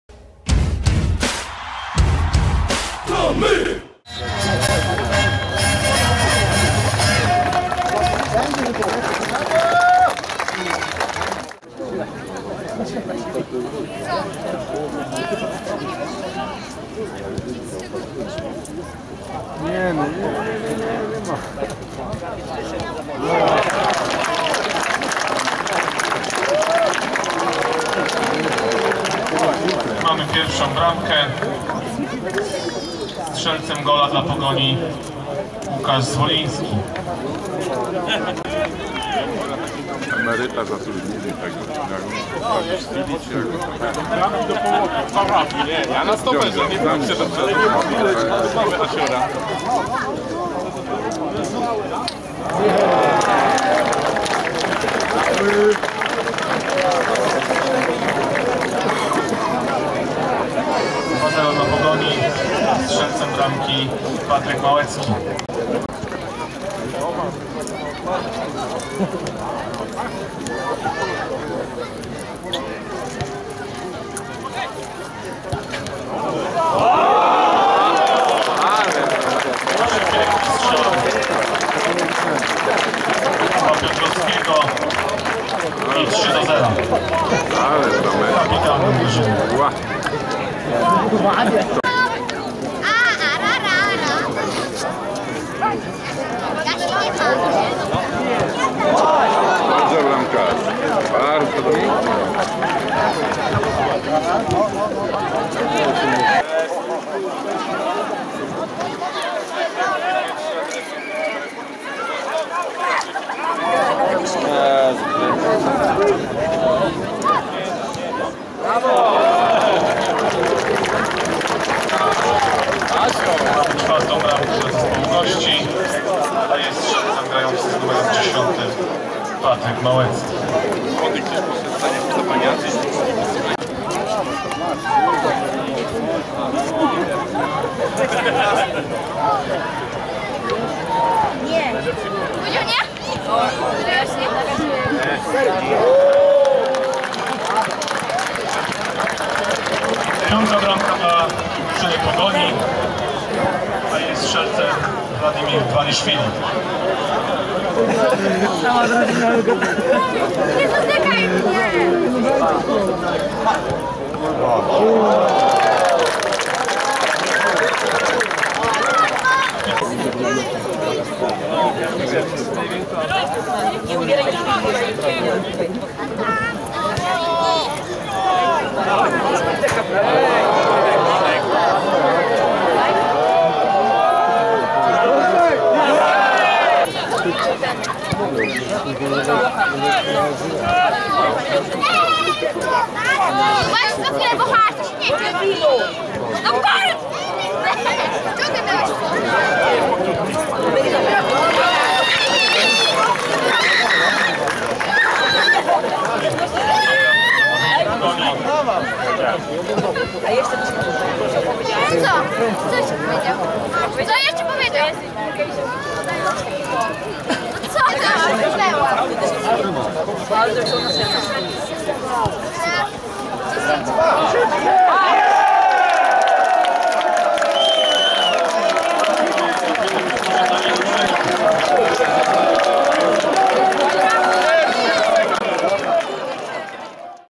Ina Goleniów 1:6 Pogoń Szczecin (SKRÓT)
Pogoń Szczecin pokonała Inę Goleniów w meczu sparingowym.

Ina Goleniów 1:6 Pogoń Szczecin

0:1 – Łukasz Zwoliński 9 min.
0:2 – Patryk Małecki 19 min.
0:3 – Jakub Piotrowski 27 min.
0:4 – Patryk Małecki 40 min.
0:5 – Wladimer Dwaliszwili 60 min.
0:6 – Karol Danielak 81 min.
1:6 – Piotr Winogrodzki 87 min.